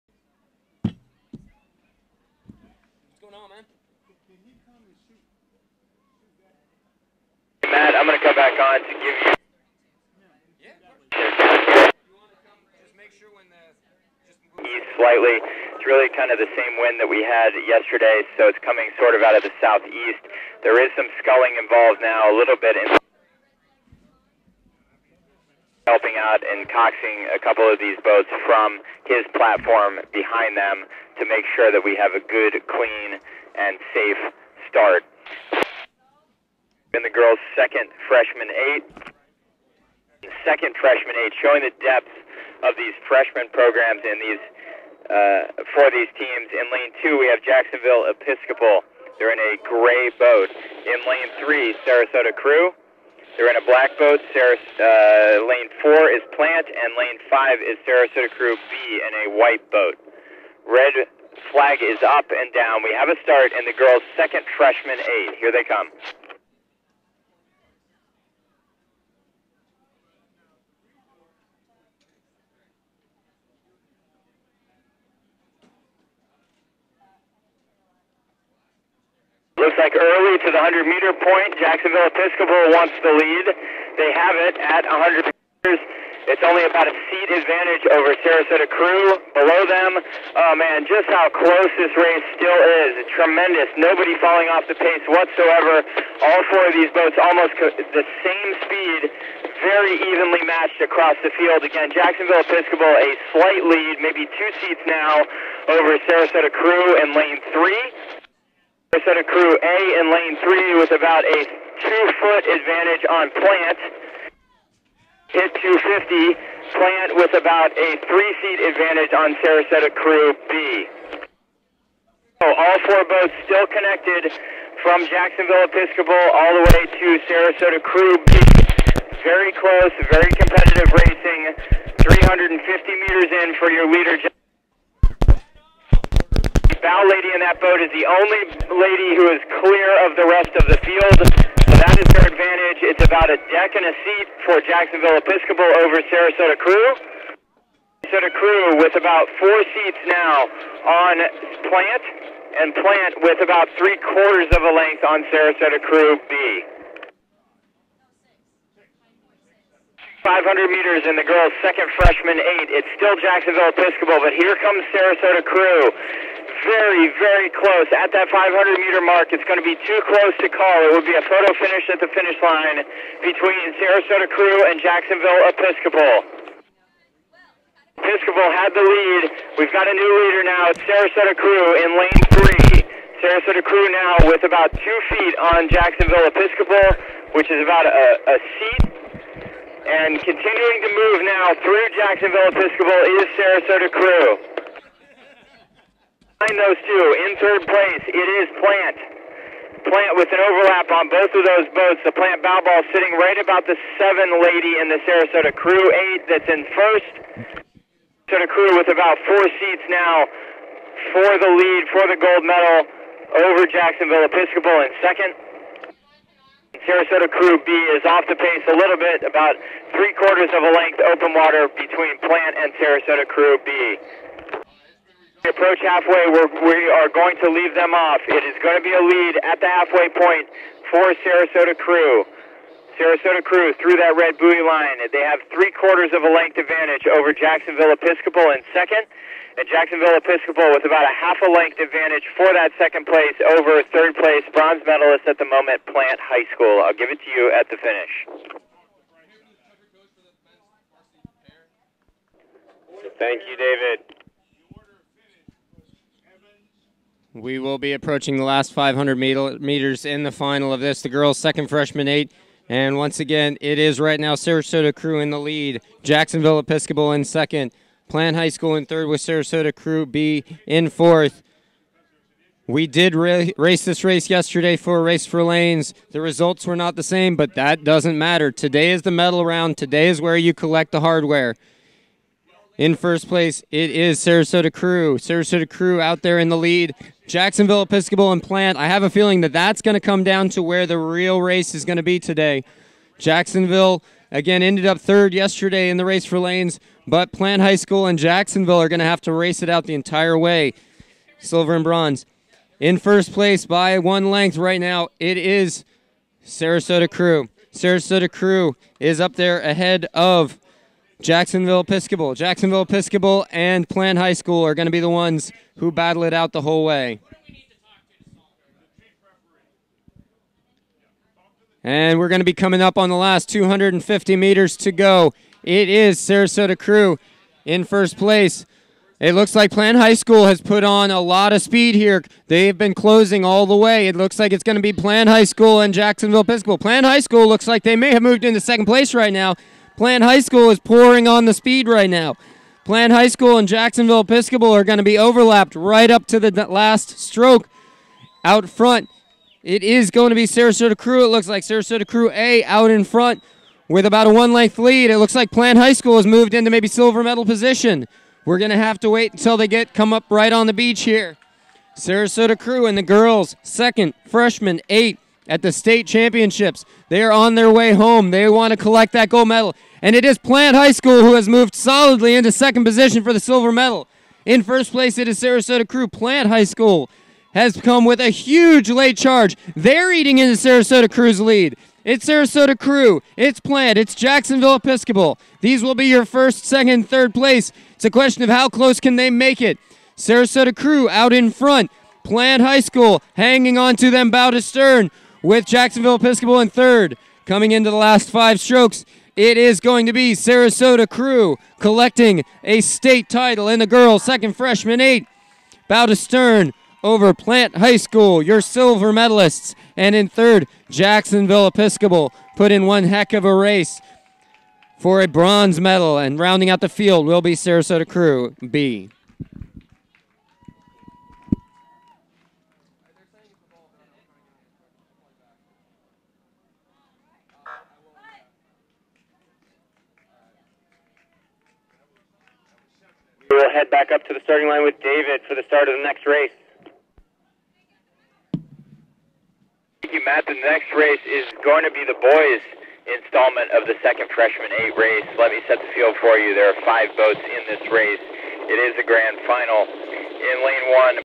2016 FSRA State Sweep Champs Girls 2nd Freshman 8+ final
Grand final with race commentary